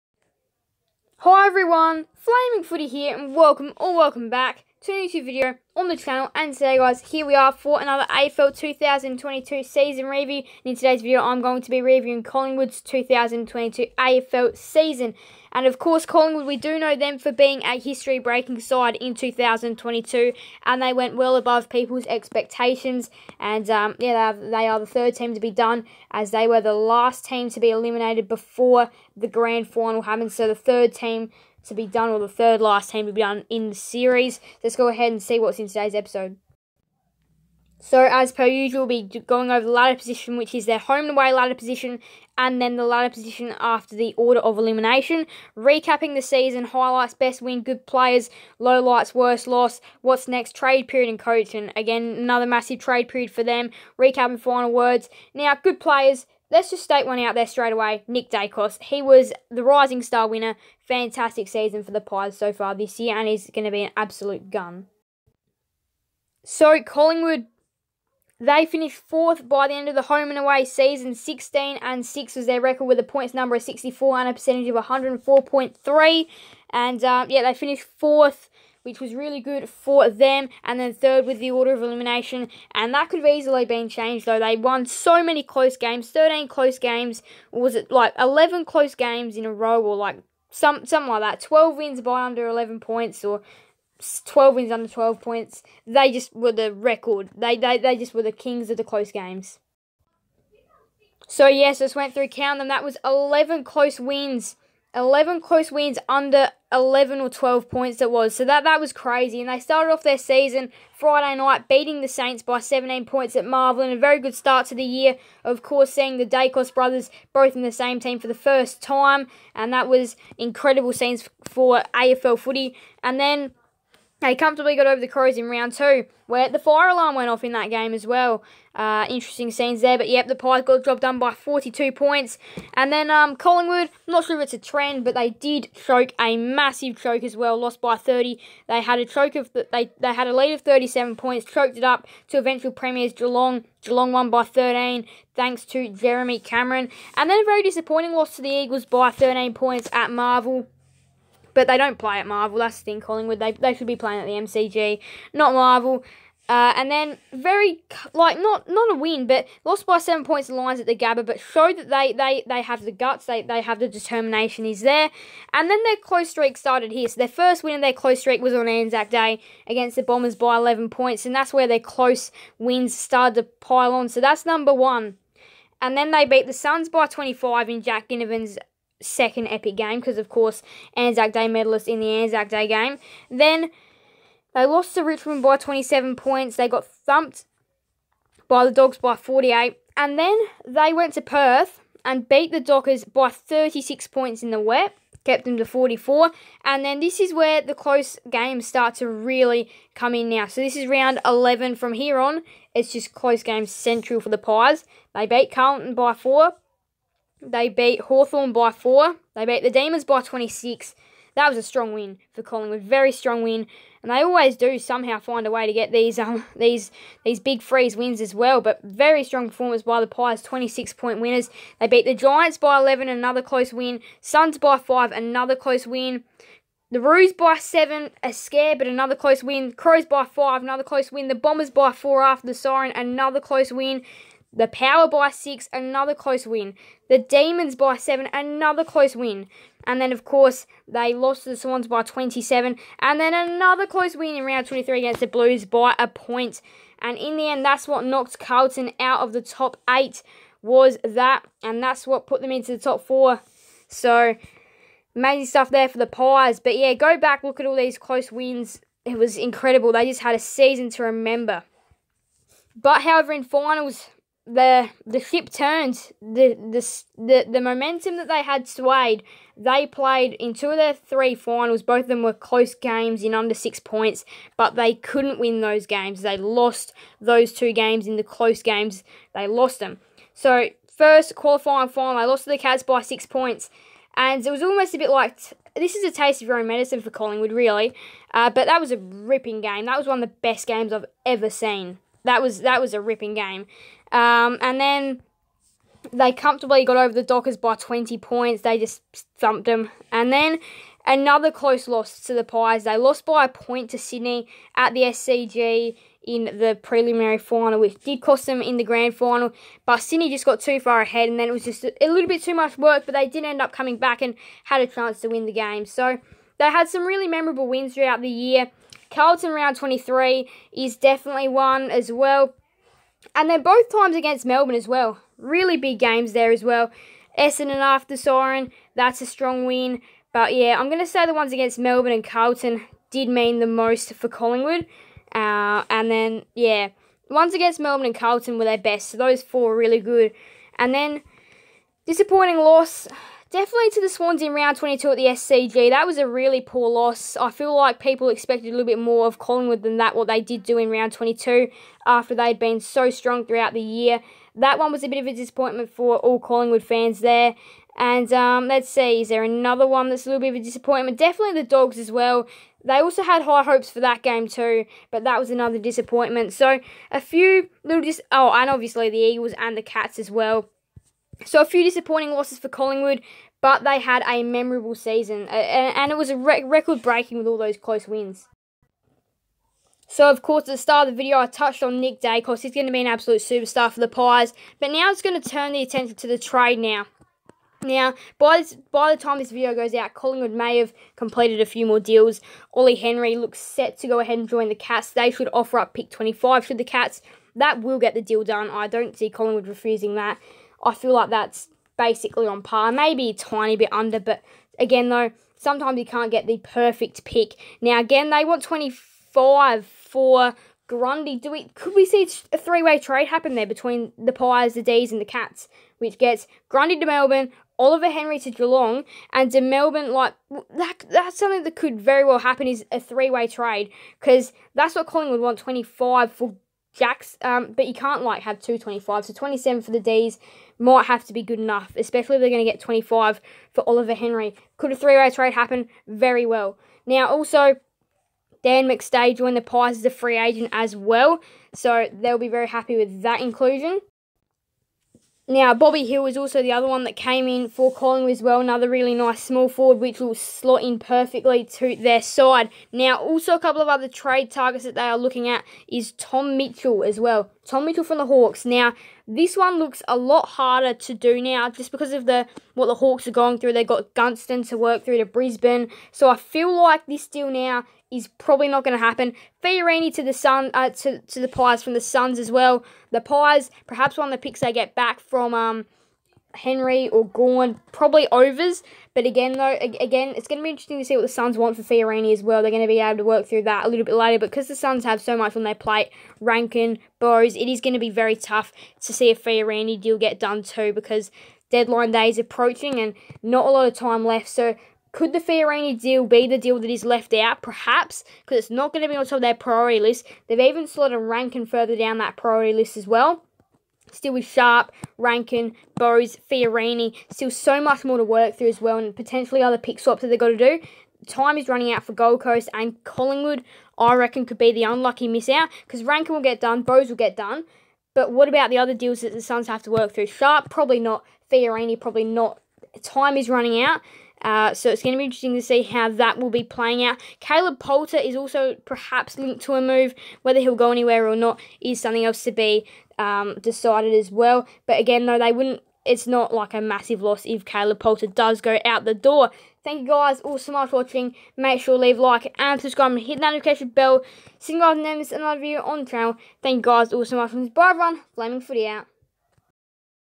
Collingwood AFL 2022 Season Review!
In todays video I continue my AFL 2022 season review series! In this series I will be doing a season review for every club. In this review I will be reviewing Collingwood's 2022 AFL Season!
Hope that you will all enjoy this new series there will be one for every team based on ladder order (for finals order of elimination)
What I Include:
Season Review (every game and ladder position)
Highlight
Lowlight
Whats Next
Reacp/Final Words